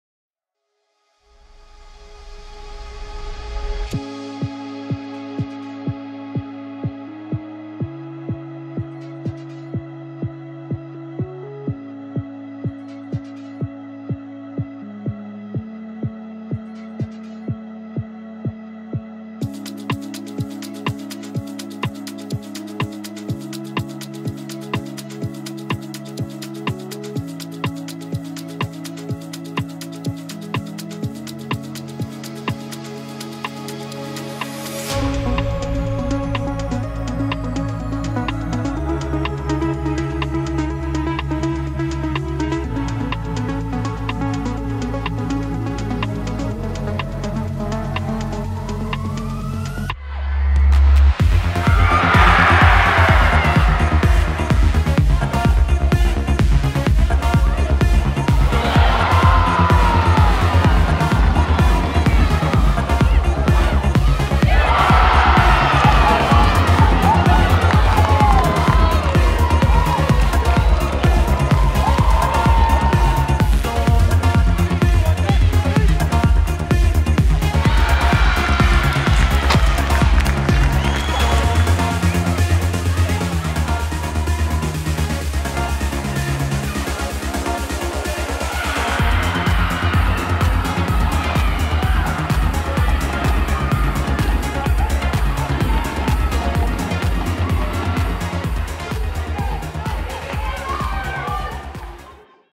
OLD FIRM | Rangers v Celtic at Broadwood | 22 Nov 2022
Rangers Women host Celtic at Broadwood Stadium this Saturday as they look to maintain their top spot in the league.